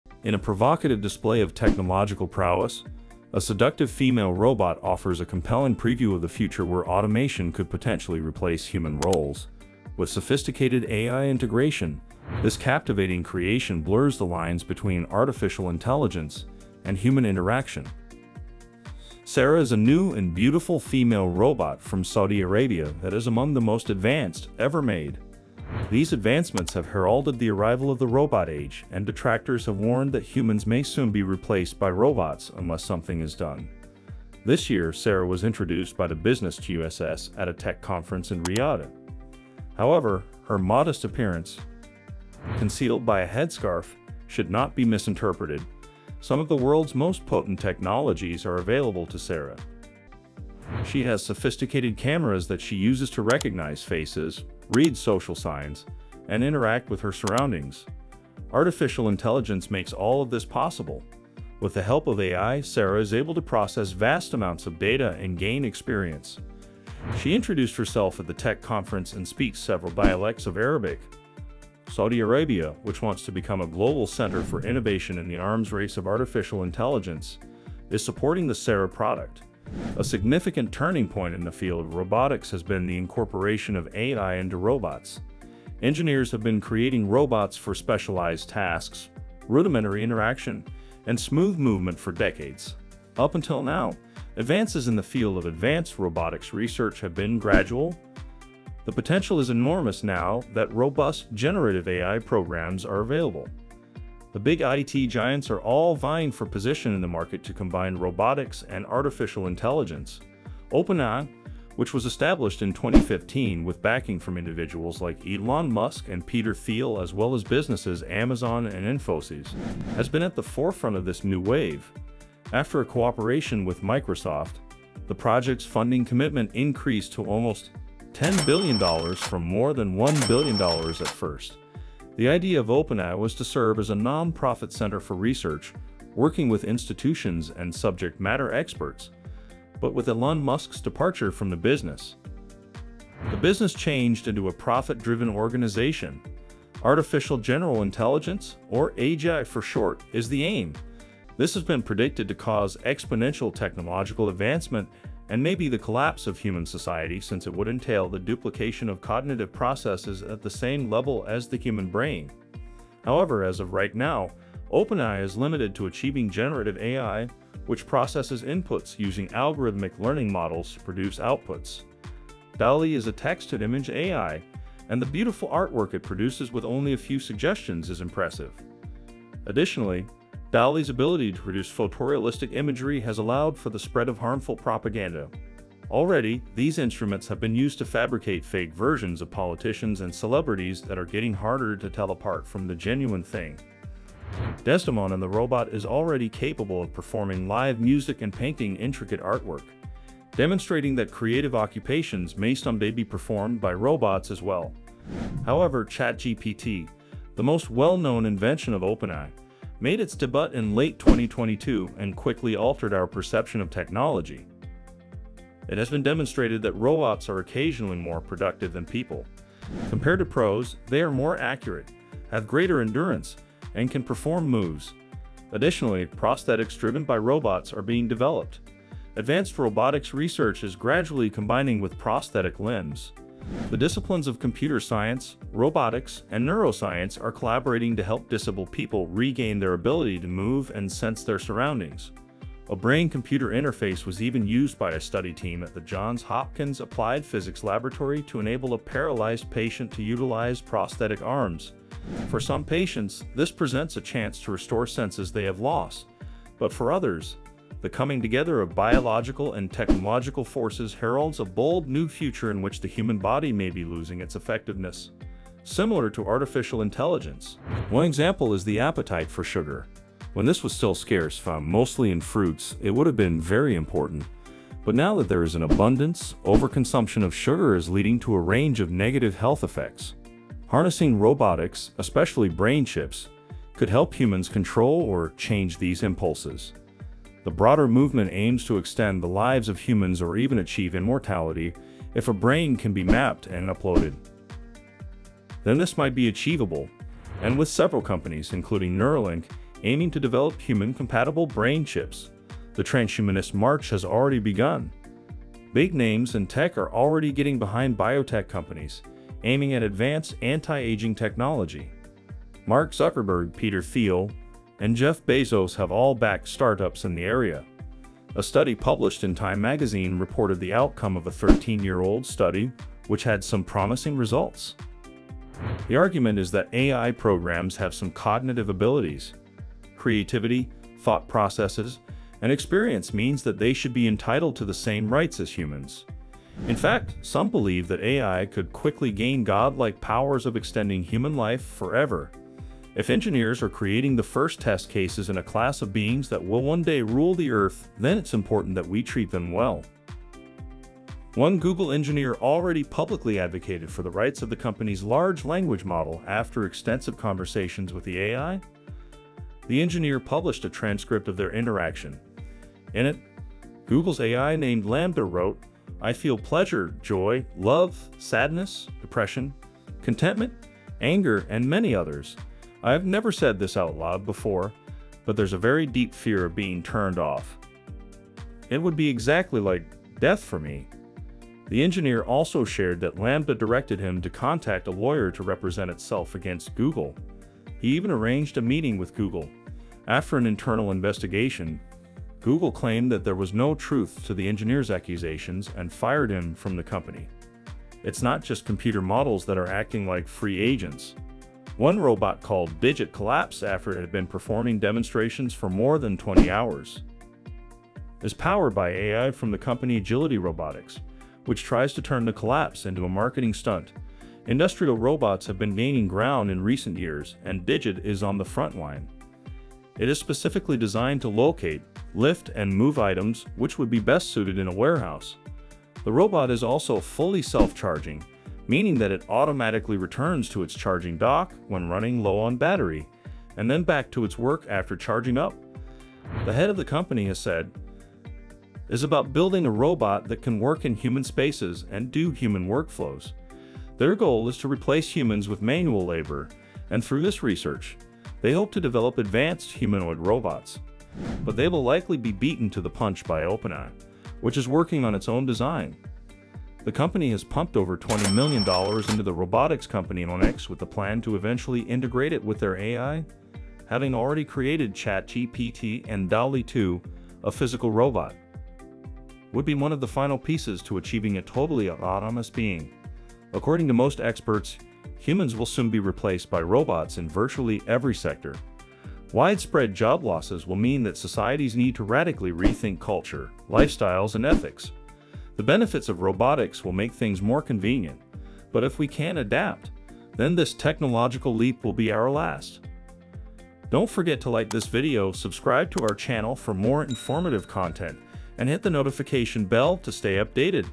INSANELY REALISTIC Robot Shows The Future! (MUST SEE)
In a provocative display of technological prowess, a seductive female robot offers a compelling preview of the future where automation could potentially replace human roles. With sophisticated AI integration, this captivating creation blurs the lines between artificial intelligence and human interaction.

About MATRIX Ai
🎨 Written, voiced and produced by MATRIX Ai